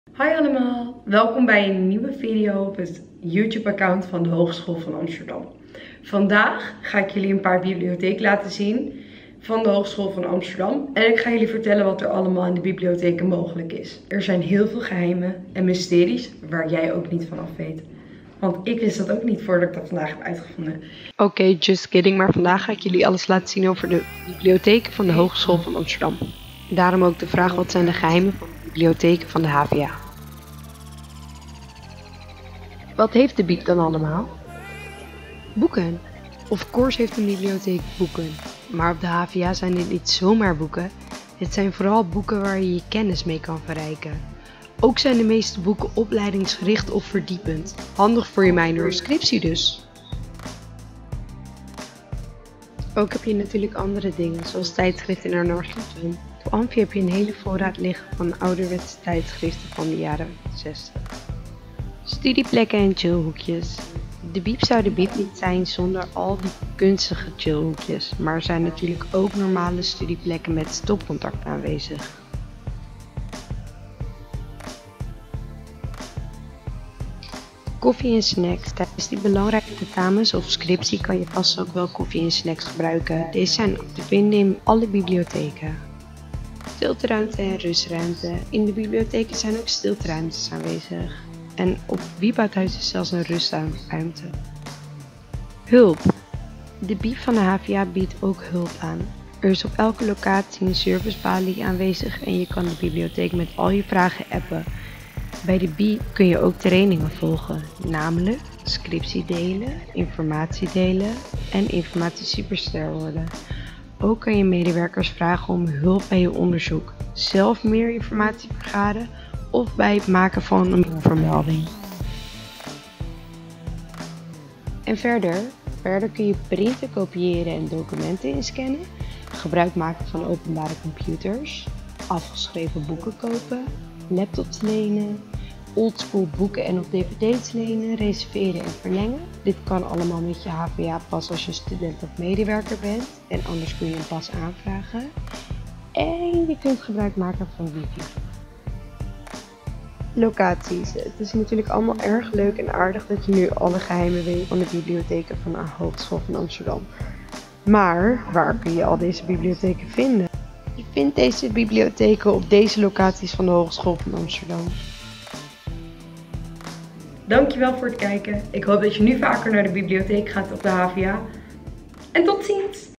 De GEHEIMEN van de HvA BIBLIOTHEEK - HvA
Dat de HvA beschikt over een bibliotheek wist je misschien al, maar heb jij enig besef over wat je allemaal in de bibliotheken van de Hogeschool van Amsterdam kan doen? Laat hieronder je vragen en/of tips weten!